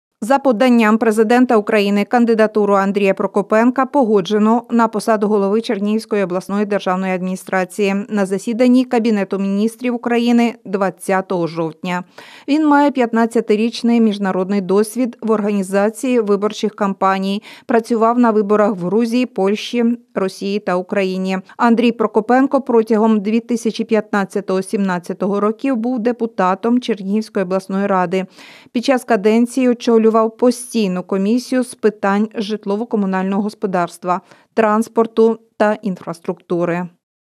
Хто стане новим губернатором Чернігівщини| Телеканал Новий Чернігів
Кабмін погодив кандидатуру Андрія Прокопенка.